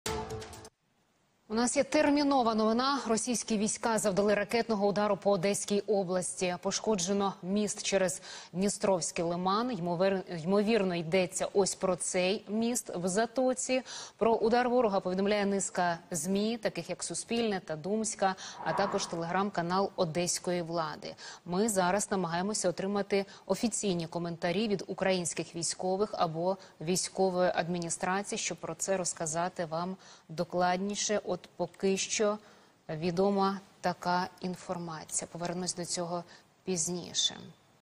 По Одещині завдали ракетного удару: пошкоджено міст через Дністровський лиман
Міст, який сполучав дорогу з Білгород-Дністровського району до Одеси, тимчасово закрили для проїзду. По ньому 26 квітня окупанти завдали ракетного удару.